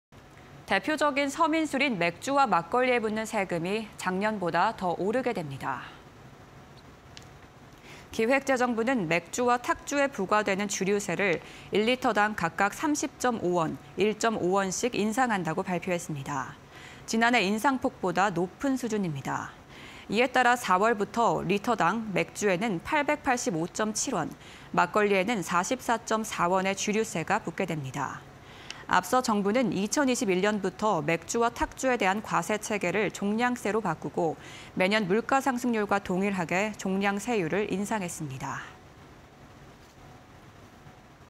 물가연동 주세에... 맥주·막걸리 세금 작년보다 더 올라 (20230119)
대표적인 서민술인 맥주와 막걸리에 붙는 세금이 작년보다 더 오르게 됩니다.

기획재정부는 맥주와 탁주(막걸리)에 부과되는 주류세를 1리터당 각각 30.5원, 1.5원씩 인상한다고 발표했습니다. 지난해 인상폭(맥주 20.8원, 막걸리 1.0원)보다 높은 수준입니다.

이에 따라 4월부터 리터당 맥주에는 885.7원, 막걸리에는 44.4원의 주류세가 붙게 됩니다.

앞서 정부는 2021년부터 맥주와 탁주에 대한 과세 체계를 종량세(주류의 양에 따라 세금을 부과)로 바꾸고 매년 물가상승률과 동일하게 종량세율을 인상했습니다.